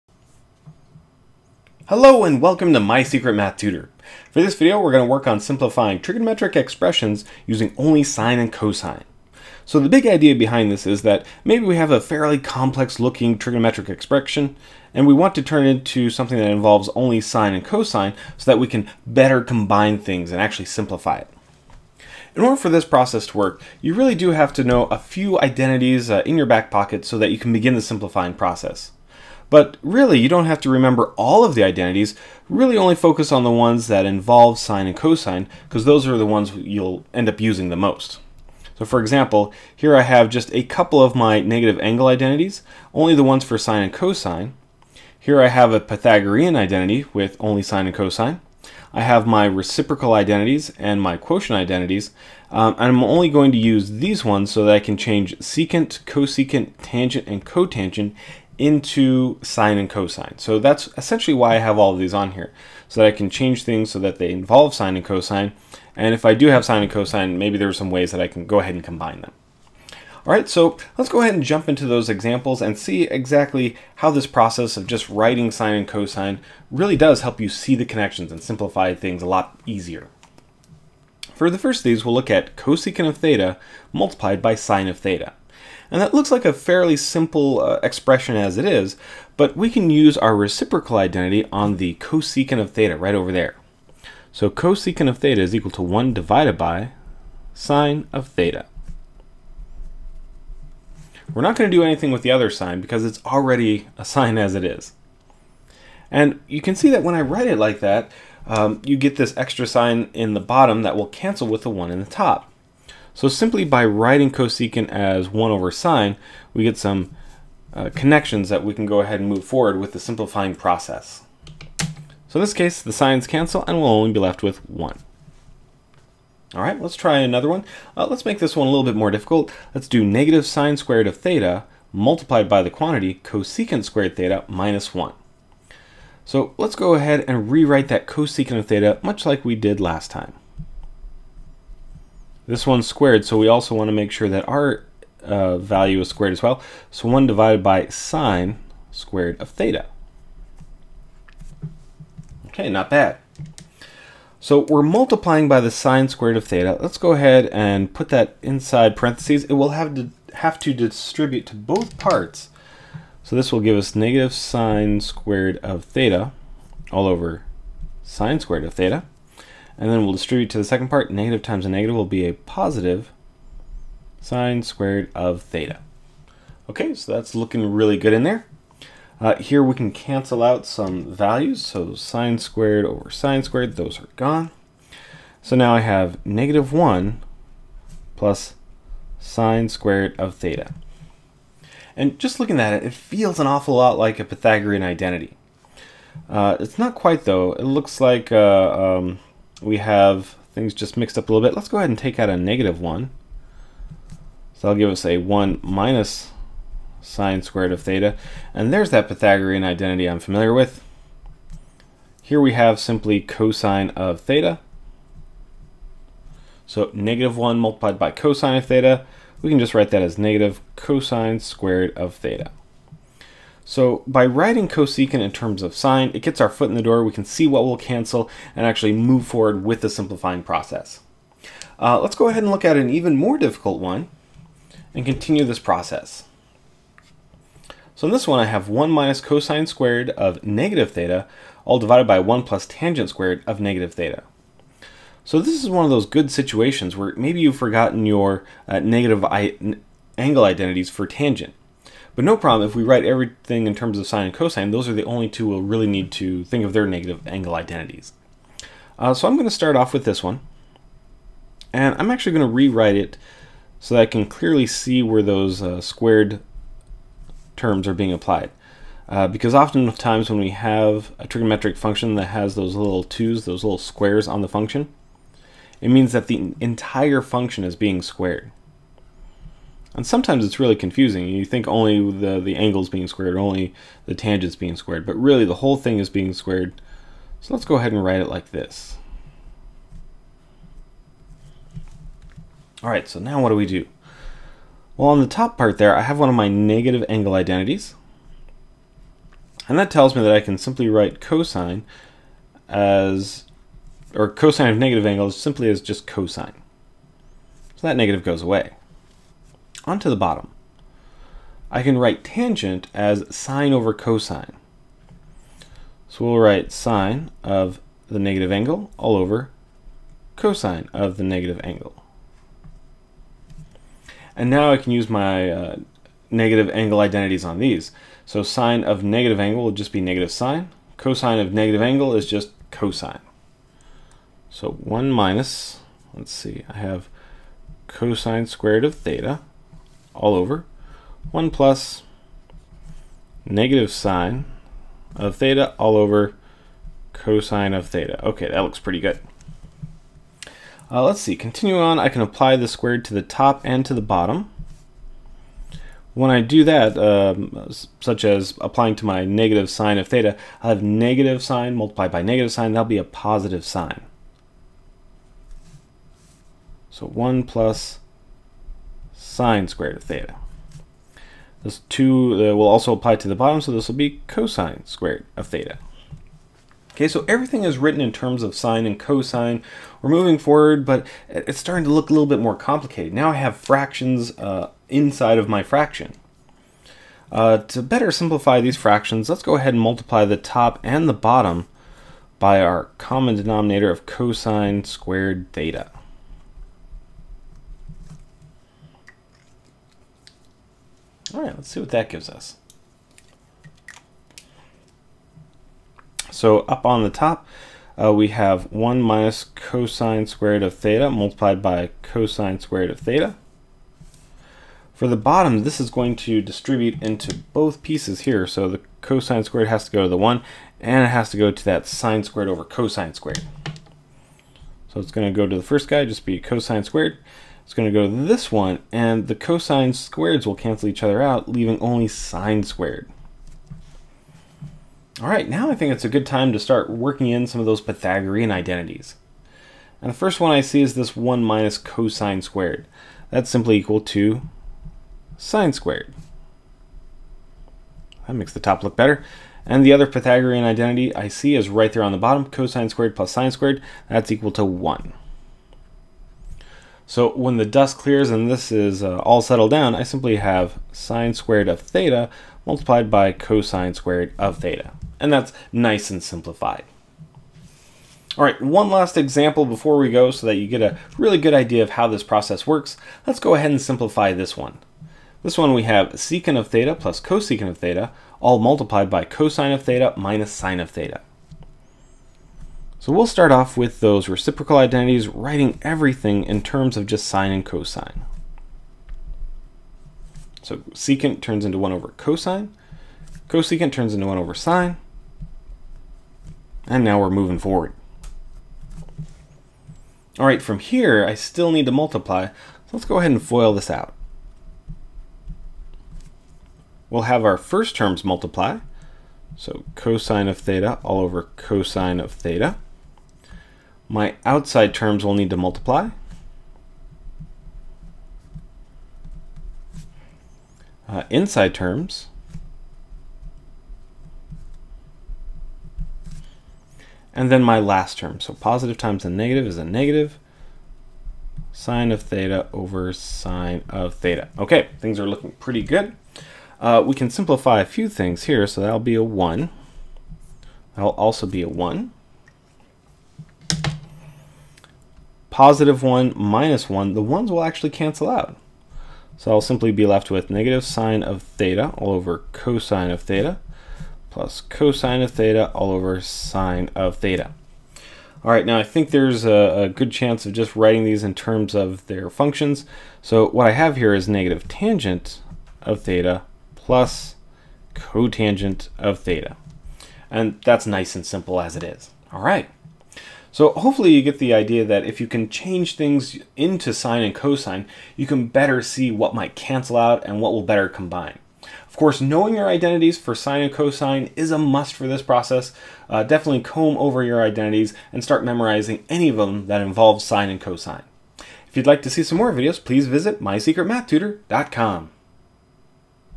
Trigonometry - How to simplify using only sin and cos
Simplifying trigonometric expressions can be tough.  With so many formulas and identities its difficult to know what to use.  Fortunately if you can change things to include only sin and cos, it cuts down on the number of formulas you have to use.  Watch this video to see how this process works and the most important formulas.